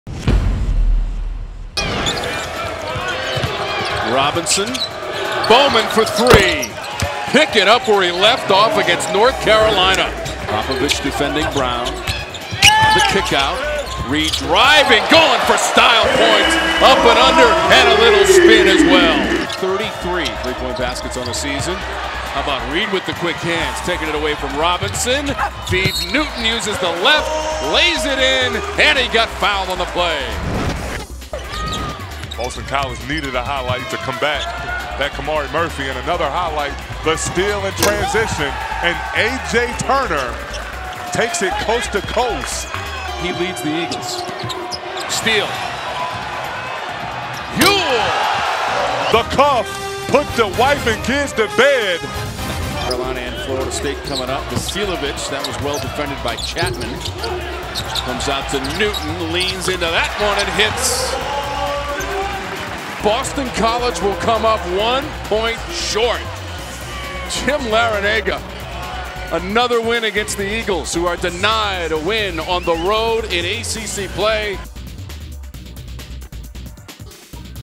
Boston College vs. Miami Men's Basketball Highlights (2016-17)
Boston College vs. Miami: The Hurricanes took care of business at home as they moved to 10-0 versus the Eagles under Jim Larranaga, 78-77. Ja'Quan Newton led all Canes scorers with 23 points. Jerome Robinson had 27 points on the night for BC.